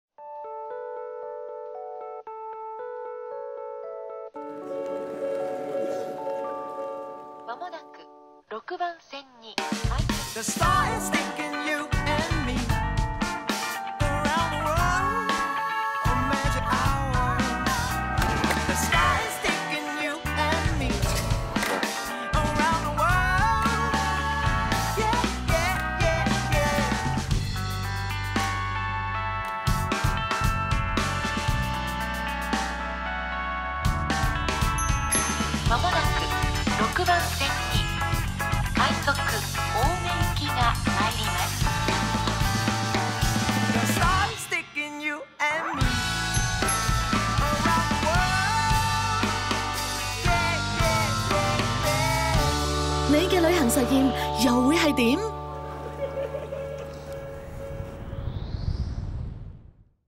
Jetstar X 觸執毛的旅行實驗 (足本長版)
觸執毛的旅行實驗︰錄低風景變成歌，到處唱
用不同的方式遊歷，才會體驗到每個地方的化學作用。
Jetstar為你帶來天天低價，讓你隨時起飛，前往超過65個目的地實現你的旅行實驗！
飛多點，感受多點！